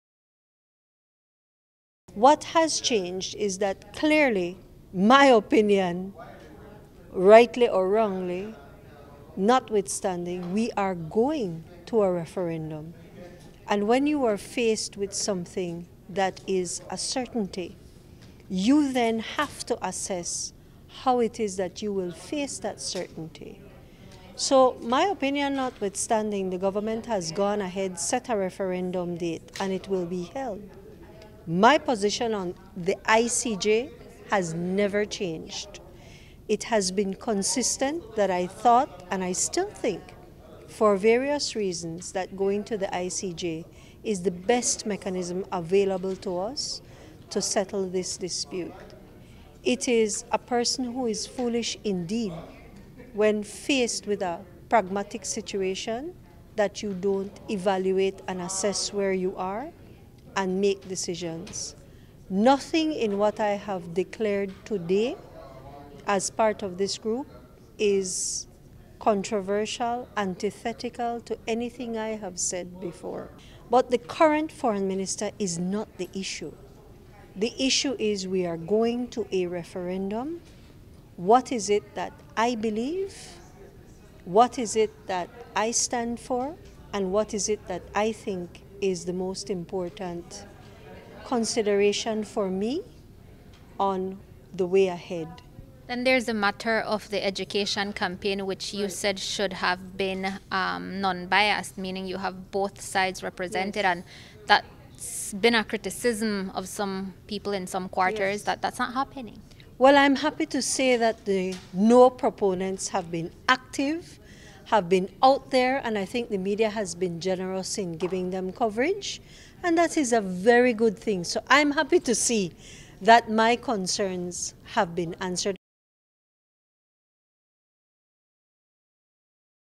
Lisa Shoman: “The current Foreign Minister is not the issue”
Attorney Lisa Shoman was Foreign Affairs Minister between June 2007 and the 2008 general elections.  While today she signed the declaration, back in April 2018, Shoman, who made her thoughts known through her Facebook profile, posted a status in which she expressed her disagreement on holding any referendum on going to the I.C.J. until certain conditions are met. Before holding a referendum, Shoman said that there must be a proper Sarstoon Protocol in place; that there is a united and non-partisan approach to any process having to do with the unfounded Guatemalan claim and until there is a Foreign Minister and government that understands and insists that Belize has internationally recognized borders. So what has changed between April of last year to today? That’s what Shoman was asked.